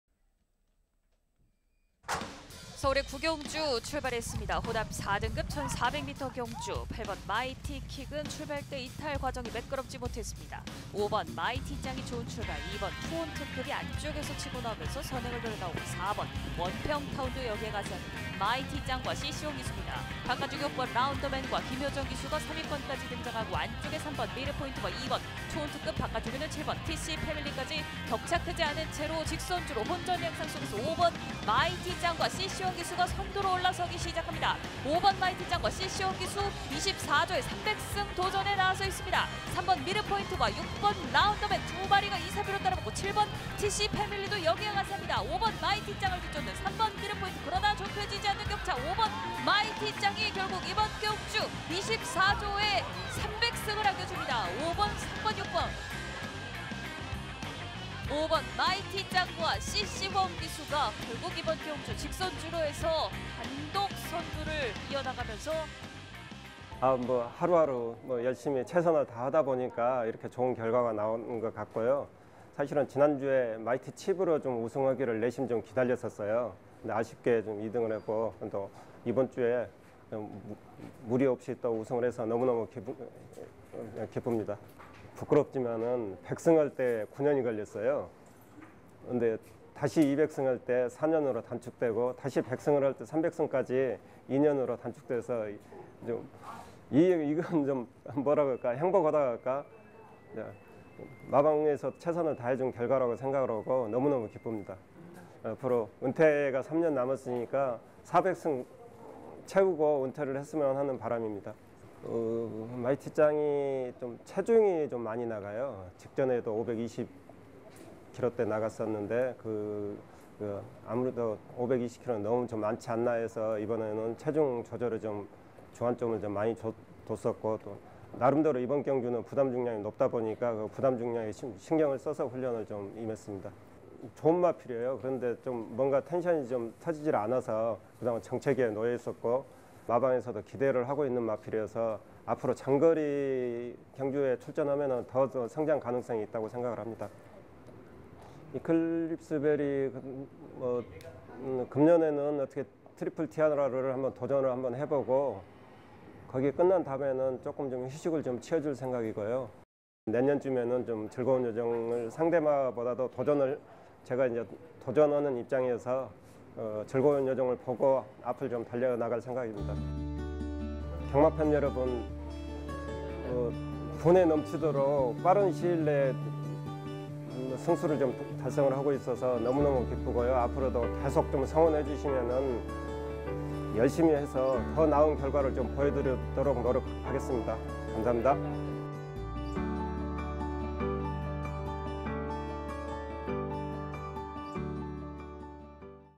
서울경마공원에서 제일 핫한 마방! 24조 수장 서홍수 조교사 300승!
서울경마공원에서 요즘 가장 주목받는 마방, 말 위에 앉아있어도 우승하는 것 같다는 말이 나올정도로 핫한 24조 서홍수 조교사! 씨씨웡 기수와  마이티짱의 호흡으로 300승을 달성했습니다. 트리플티아라 삼관을 도전하는 이클립스베리의 1관문, 2관문 우승과 함께 축하합니다~

본 영상은 비상업·상업적 사용이 불가하며